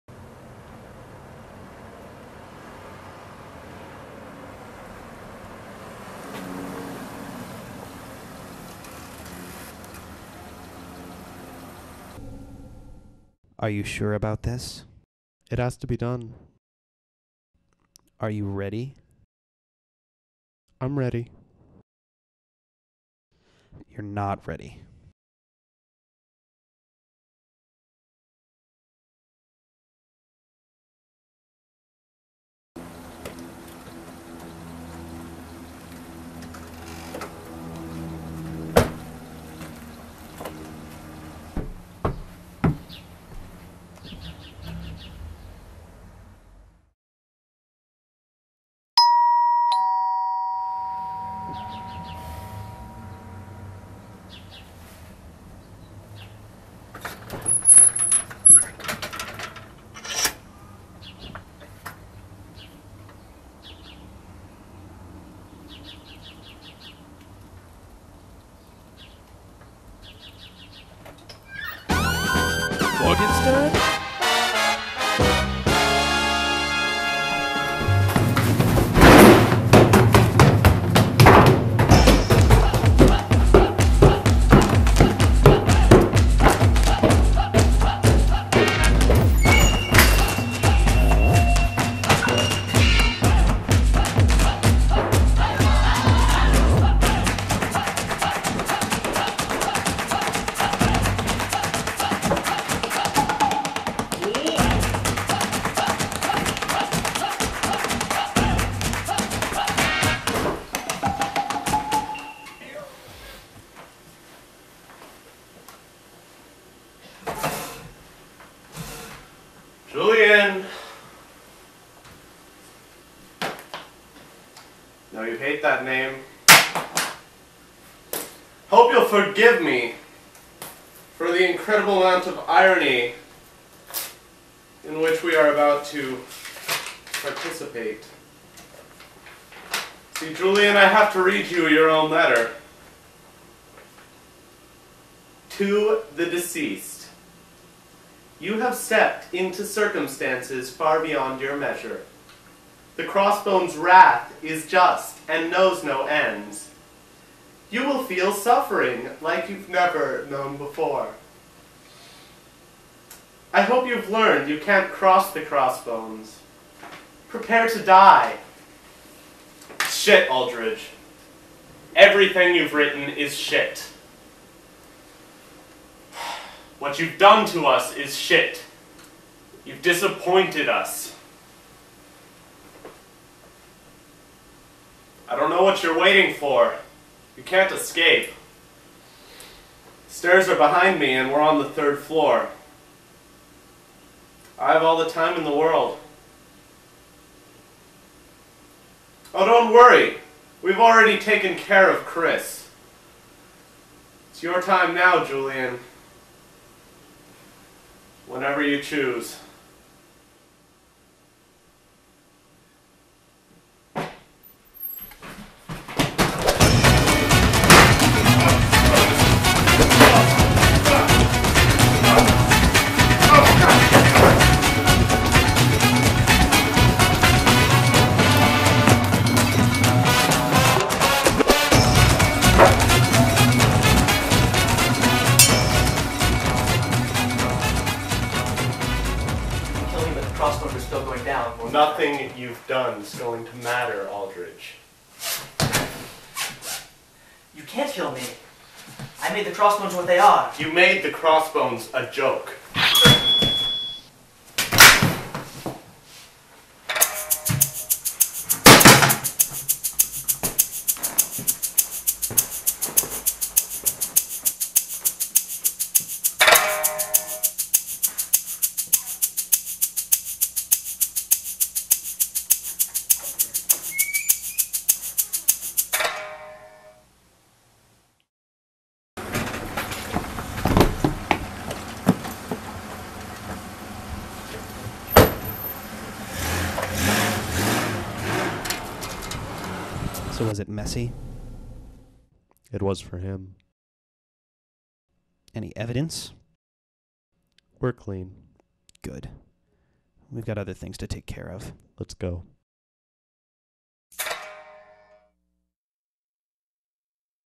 Lucius Meets Up
Lucius has a date... WITH DESTINY.

-- Created for the 2007 ARG "Crossbones" --

Lucius Morgenstern: Joseph Strong
J. Chris Aldridge: Adam Nakama
Sam Williams: Rick Desilets

Directed by: Adam Nakama, Rick Desilets